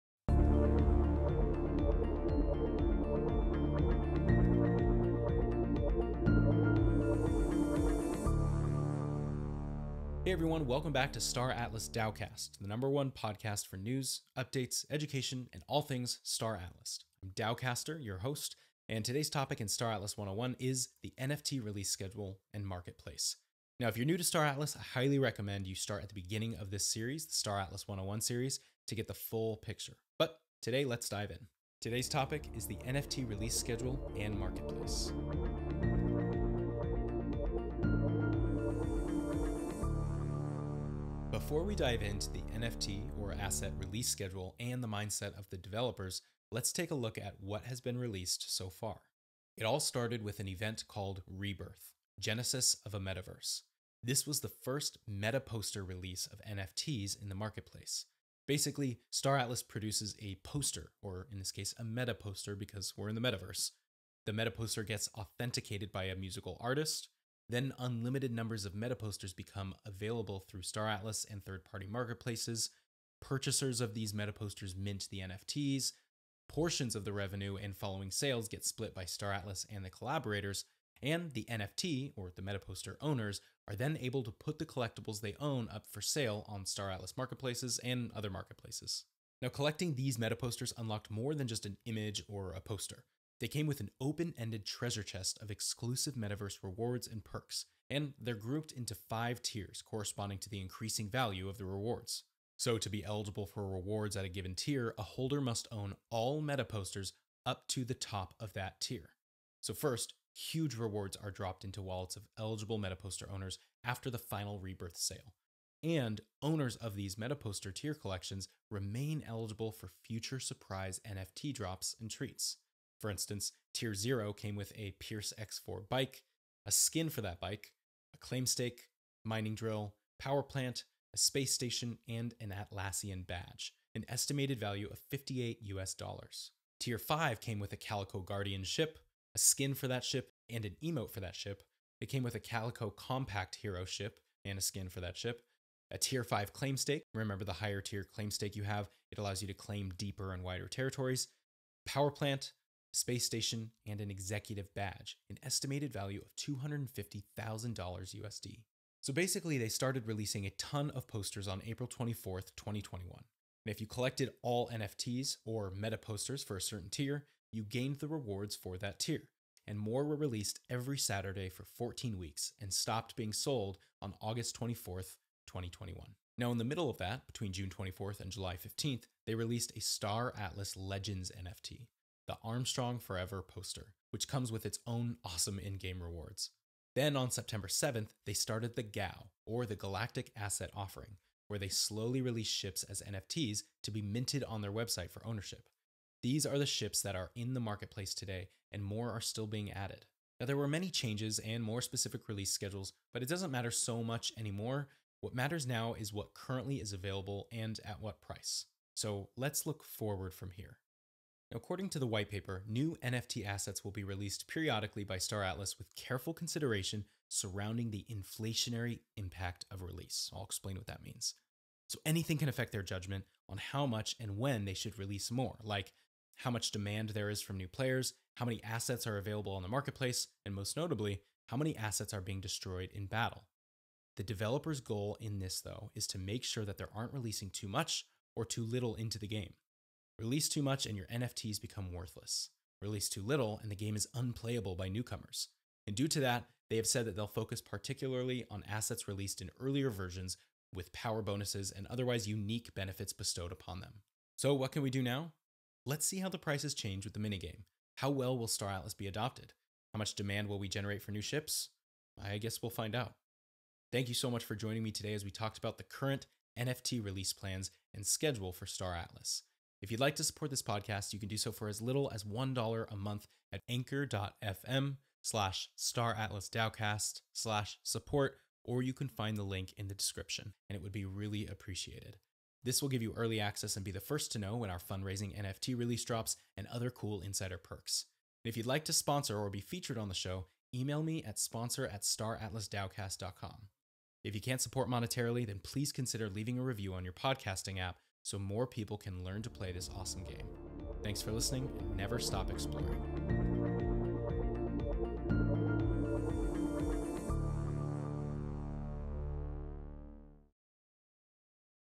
Star Atlas NFT Release Schedule & Marketplace | Star Atlas 101
Before we dive into the NFT or Asset release schedule and mindset of the developers, let’s take a look at what has been released so far.

It all started with an event called ReBirth: Genesis of a Metaverse. This was the first Metaposter Release of NFTs in the marketplace. Basically, Star Atlas produces a Meta-Poster, The Meta-Poster gets authenticated by a music artist/XR artist, then Unlimited numbers of the Meta-Posters became available through Star Atlas and 3rd party marketplaces, Purchasers of the Meta-Posters mint the NFTs, Portions of the revenue and following sales gets split by Star Atlas and collaborators and the NFT ( or Meta-Poster) owners are then able to put the collectibles they own up for sale at Star Atlas or any other marketplaces.

Collecting these Meta-Posters unlocked more than just an image of a trading card, they came with an open-ended treasure chest of exclusive metaverse rewards and perks. And they are grouped into five Tiers corresponding to the increasing value of the rewards.

So, To be eligible for rewards of a given Tier, a holder must own all Meta-Posters up to the top of that Tier. So first,  huge rewards are dropped into wallets of eligible Meta-Poster owners after the final ReBirth sale, AND Owners of Meta-Poster Tier collections remain eligible for future surprise NFT drops and treats.

For instance, Tier 0 came with a Pearce X4 Bike, a skin for the bike, a claim stake, mining drill, power plant, and an orbital space station, and an Atlassian badge, an estimated value of $58 US Dollars.

Tier 5 came with a Calico Guardian, a skin for the ship, an emote for the ship, A Calico Compakt Hero, and a skin for that ship, a Tier 5 Claim Stake (remember that a higher tier claim stake allows you to claim deeper and wider territories), a power plant, a space station, and an Executive badge, an estimate value of $250,000 USD.

So basically they started releasing a ton of posters on April 24th, 2021. If you collected all NFTs or meta poster for a certain tier, you gained the rewards for that tier. More were released every Saturday for 14 weeks and stopped being sold on August 24th, 2021.

Now in the middle of that, between June 24 and July 15, they released a Star Atlas Legends NFT, the Armstrong Forever Poster, which comes with it’s own awesome in-game rewards.

Then on Spetember 7th, they started the GAO, or the Galactic Asset Offering, where they slowly released ships as NFTs to be minted on their website for ownership. These are the ships that are in the marketplace today and more are still being added.

There were many changes and more specific release schedules, but it doesn’t matter so much anymore. What matter now is what is currently available at what price. But let’s look forward.

According to the Whitepaper, “New NFT assets will be released periodically by Star Atlas with careful consideration surrounding the inflationary impact of release.

So anything can affect their judgement on how much and when they should release more, like how much demand there is from new players, how many assets are available on the marketplace, and most notably, how many assets are being destroyed in battle.?

The developers goal in this though is to make sure that they aren’t releasing too much or too little into the game. Release too much and your NFTs become worthless. Release to little and the game is unplayable by newcomers.

Due to that, they have said that they’ll focus particularly on assets released in earlier versions with power bonuses or otherwise unique benefits bestowed upon them.

So what can we do now? Let;s see how the prices change with the mini game. How well will Star Atlas be adopted? How much demand will we generate for new ships? I guess we’ll find out!